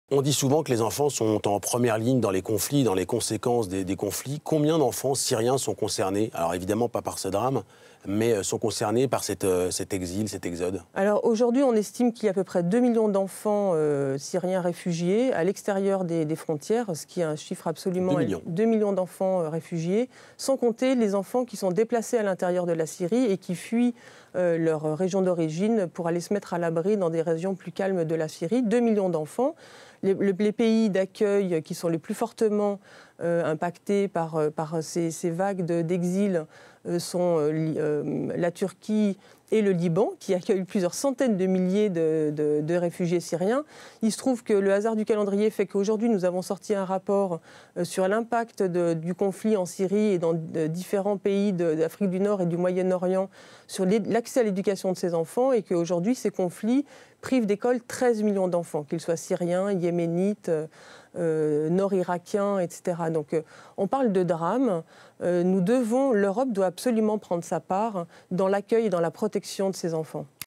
Bénédicte Jeannerod d'Unicef France: "à peu près 2 millions d'enfants syriens réfugiés"
photo d'un enfant syrien mort sur une plage de Turquie qui a créé un véritable choc dans toute l'Europe. Interrogée sur le nombre de mineurs syriens qui fuient les conflits, elle a répondu: "Aujourd'hui, on estime qu'il y a à peu près 2 millions d'enfants syriens réfugiés à l'extérieur des frontières. C'est sans compter les enfants qui sont déplacés à l'intérieur de la Syrie et qui fuient leur région d'origine pour aller se mettre à l'abri dans des régions plus calmes de Syrie. Les pays d'accueil qui sont les plus fortement impactés par ces vagues d'exil sont la Turquie et le Liban qui a eu plusieurs centaines de milliers de réfugiés syriens".